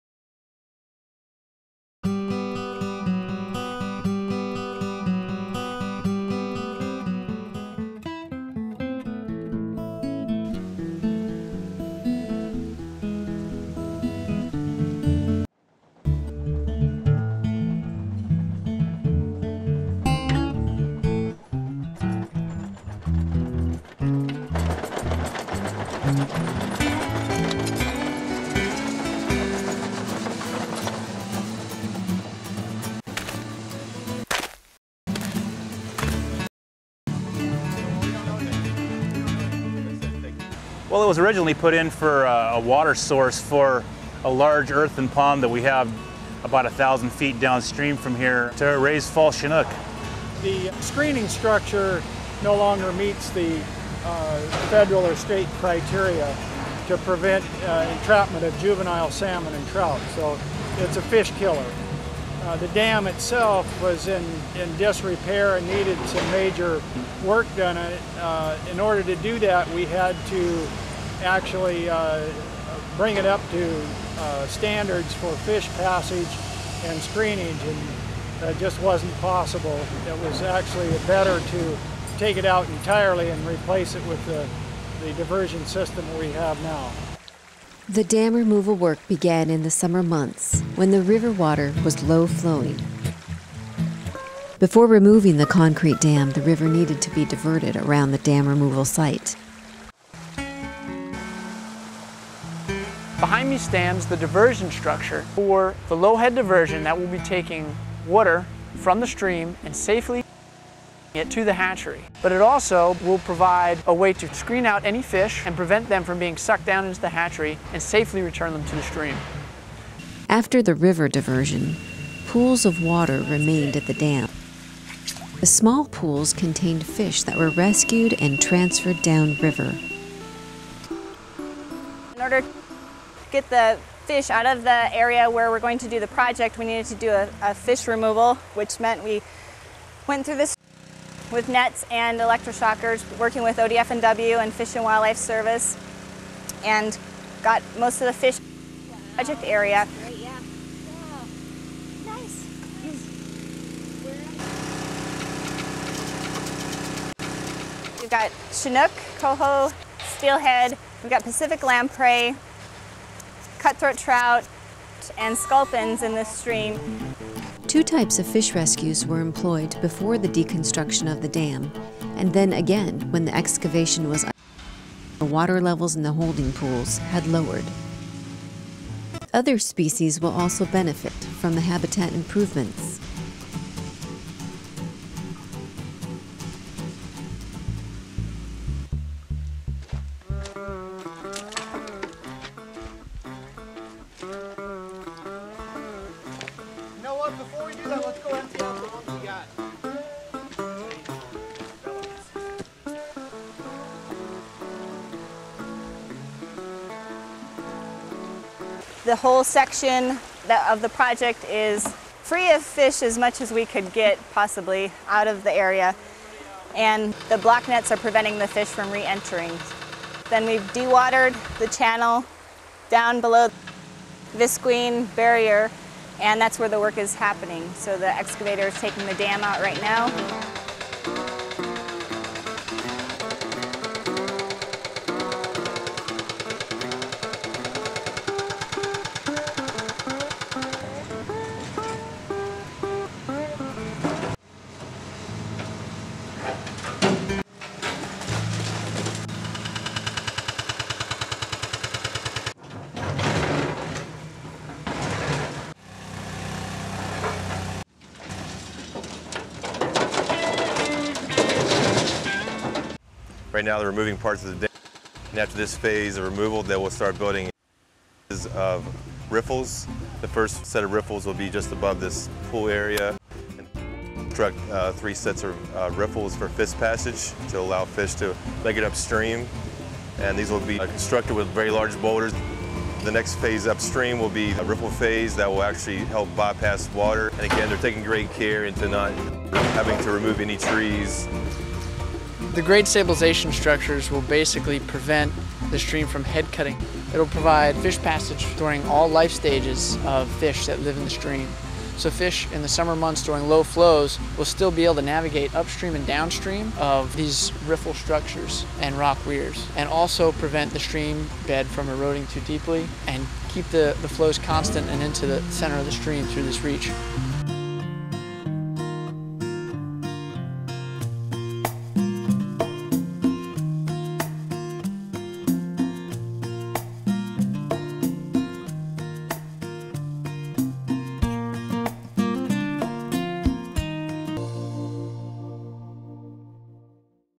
Dam Removal on the Klaskanine
Clatsop County, Oregon Department of Fish and Wildlife, NOAA and others partnered for this dam removal on the Klaskanine River in Oregon to allow for fish passage.